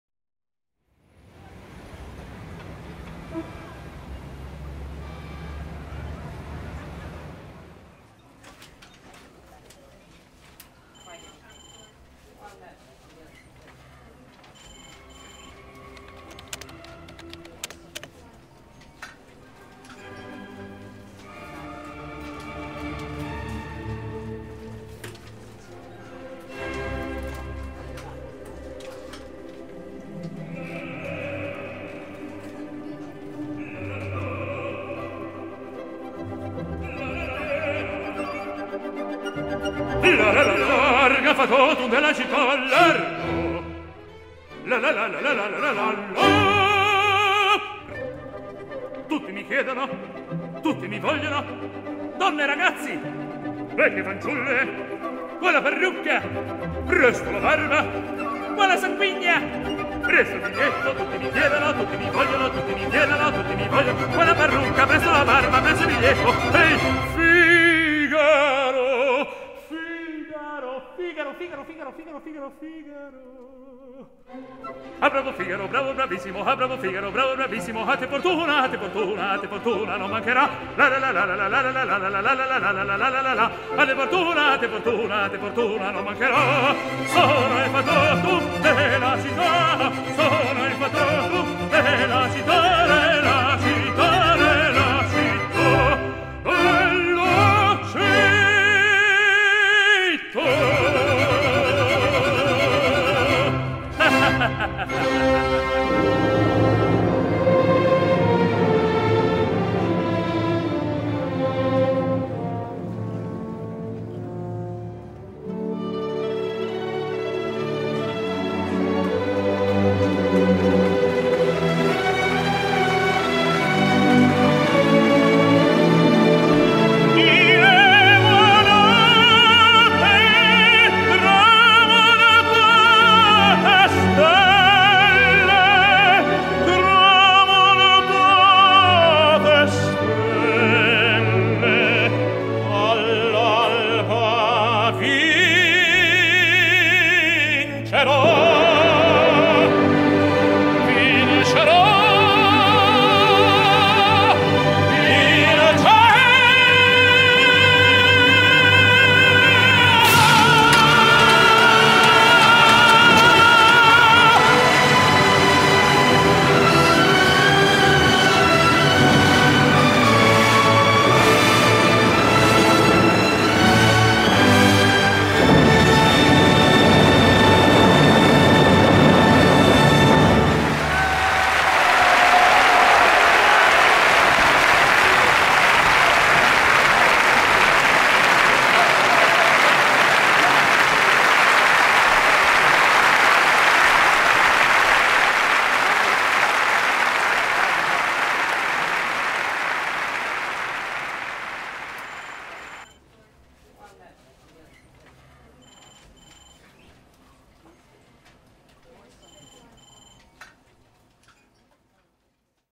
OperaMan
A funny illustration to show a former opera star turned to office cubicle worker.